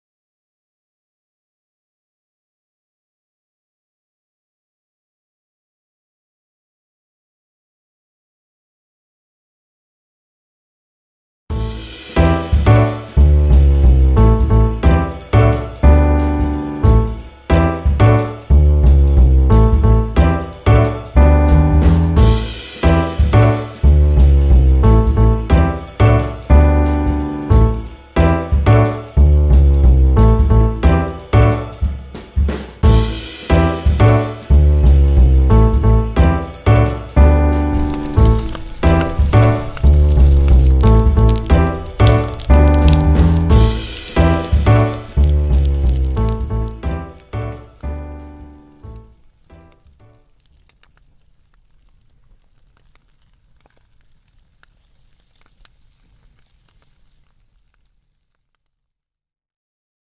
TSBBQホットサンドを使ってみた
トマトを切って、ホットサンド作り！